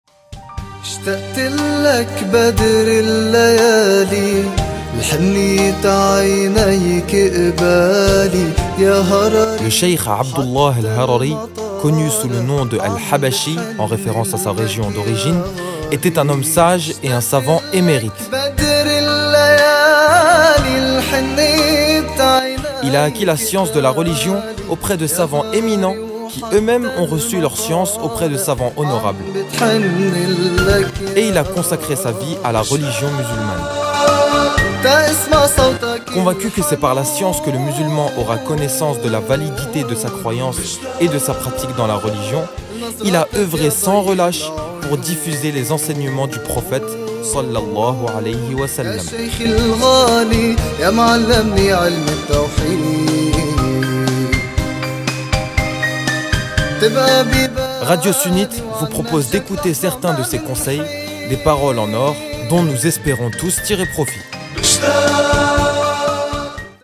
PAROLES EN OR du chaykh عبد الله الهرري ^Abdoullah Al-Harariyy
✨PAROLES EN OR ✨ tous les jours au Programme de Radio Sunnite pour ce mois béni de Ramadan

Le Chaykh عبد الله الهرري ^Abdoullah Al-Harariyy connu sous le nom de Al-Habachiyy en référence à sa région d’origine, était un homme sage et un savant émérite. Il a acquis la science de la religion auprès de savants éminents qui eux-mêmes ont reçu leur science auprès de savants honorables et il a consacré sa vie à la religion musulmane. Convaincu que c’est par la science que le musulman aura connaissance de la validité de sa croyance et de sa pratique dans la religion, il a œuvré sans relâche pour diffuser les enseignements du Prophète صلى الله عليه وسلم.
Radio sunnite vous propose d’écouter certains de ses conseils, des Paroles en or dont nous espérons tous tirer profit.